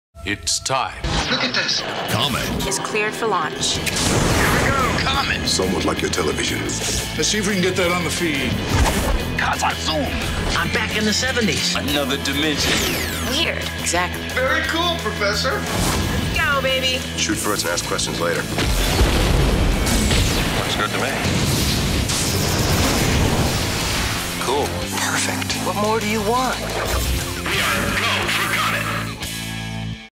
COMET Science Fiction Channel
COMET is a new television channel dedicated to sci-fi entertainment offering popular favorites, cult classics, and undiscovered gems, every day. Watch COMET and Space Out.